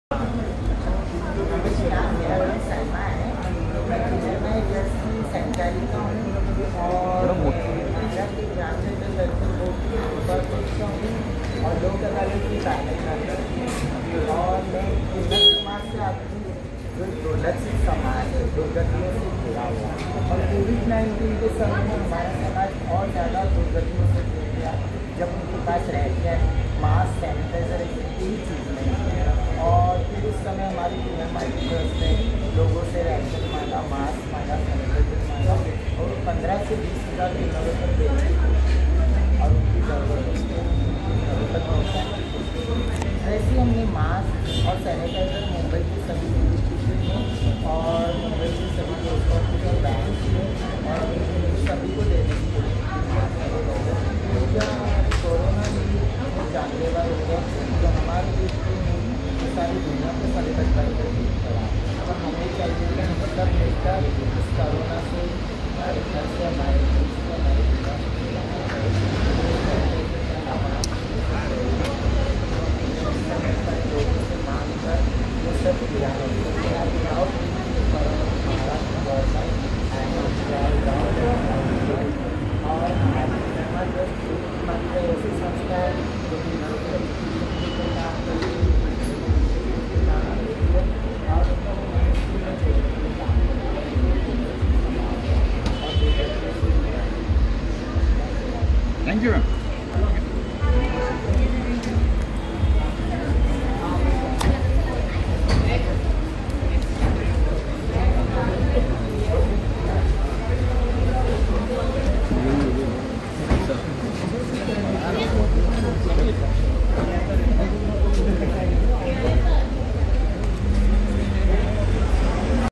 Kinnar Maa Trust Media Coverage Dadasaheb Phalke IconAwardFilm Covid19Yoddha2020 awarded to SalmaMam
Today 23rd November, 2020 Dadasaheb Phalke Icon Award Film Covid19 Yoddha 2020 awarded to our President Ms. Salma Khan congratulations at Peninsula Grand Hotel, Mumbai. During award ceremony media coverage for sensitizing mainstream and stakeholders about issues and need of Transgender and our support work during Covid19 pandemic. 🙏🤲😷